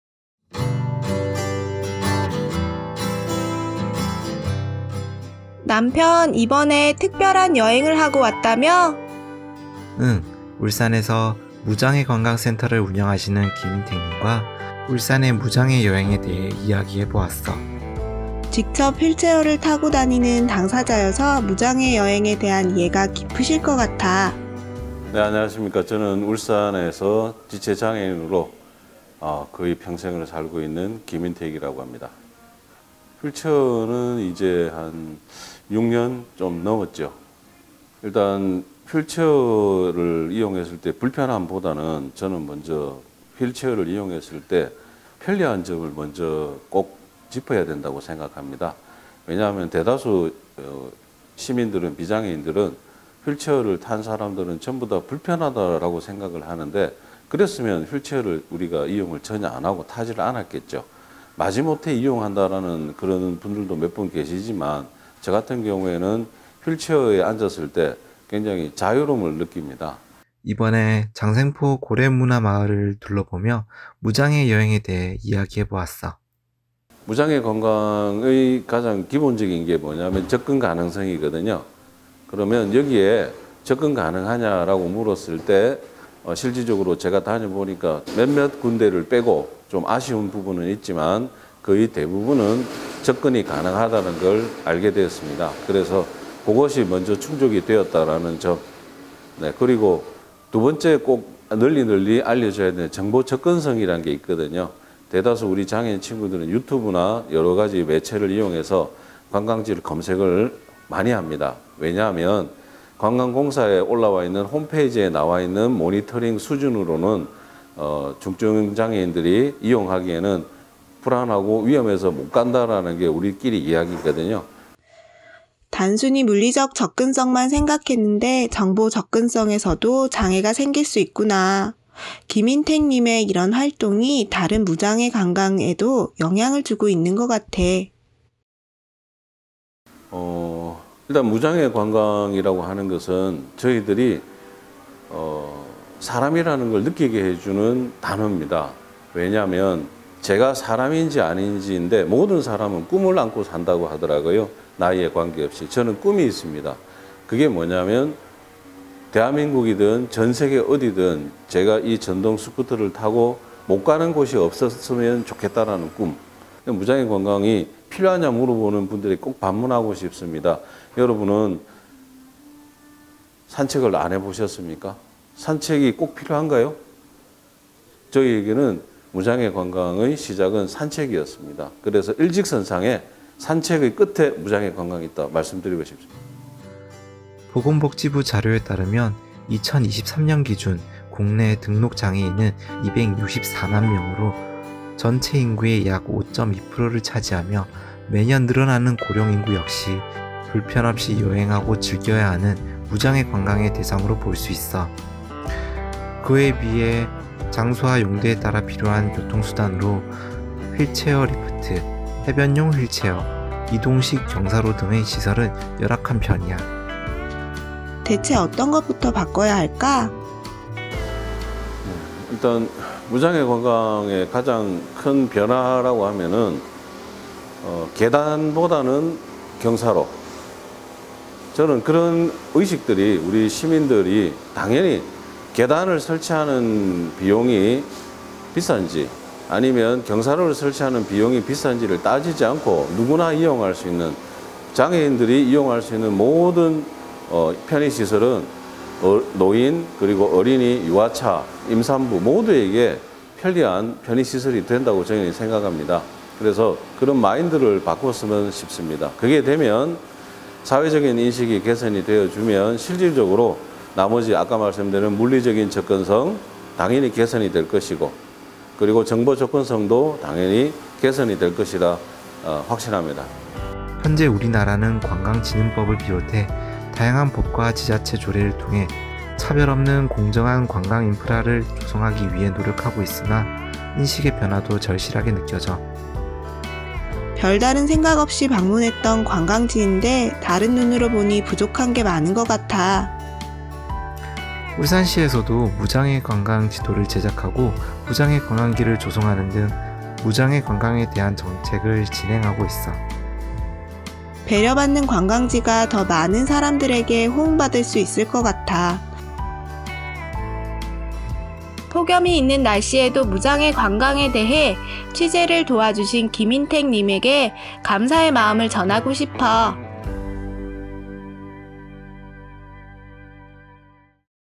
[2024 별별기자단 슈슈] Let's GO 무장애 관광!
*위 영상은 국가인권위원회 별별기자단 제작 영상으로 국가인권위원회 공식입장과 다를 수 있습니다.

안녕하세요. 2024 국가인권위원회 별별기자단 슈슈 입니다.

울산 무장애관광센터를 운영하시는 김인택님과 장성포 고래마을을 둘러보며 무장애 여행에 대해 알아보았습니다.  휠체어를 이용하시는 당사자분과 함께 하는 무장애 여행으로  관광지의 접근 가능성과 정보 접근성이 매우 중요하다는 사실을 알게 되었습니다.  누구나 불편없이 어디든 여행할 수 있는 세상이 되었으면 합니다.

그럼 팀 슈슈와 함께 무장애 관광에 대해 알아볼까요?